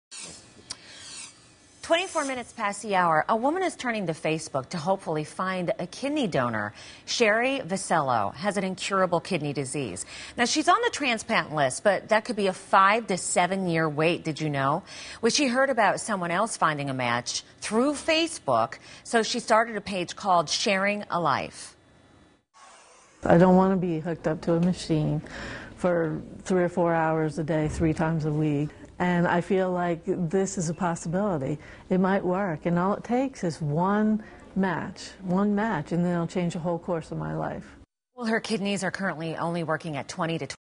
Turning to Facebook for a kidney
Facing dialysis 3 days a week and a 5 year transplant waiting list, a woman has started a Facebook page to find a donor.